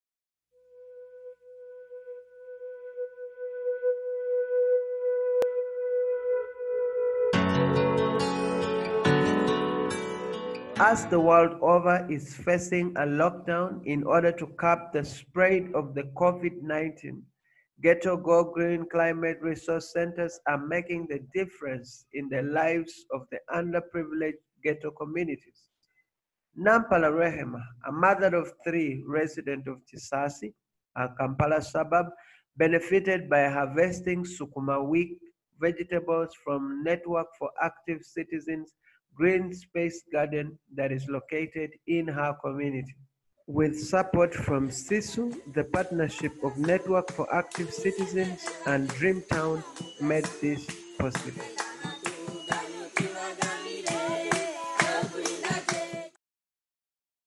GREEN GHETTOS: A Beacon Of Hope in COVID - 19 Lockdown
With the current situation of the country's lockdown due to the corona crisis, the green spaces and urban gardens that have been developed as part of the project in the beginning of 2020 are a beacon of hope to vulnerable communities in the ghettos by providing their communities with vegetables, and some fruits.
Ghetto Go Green is a climate change campaign that promotes youth led urban adaptation mechanisms in the ghetto communities of Kampala, Uganda.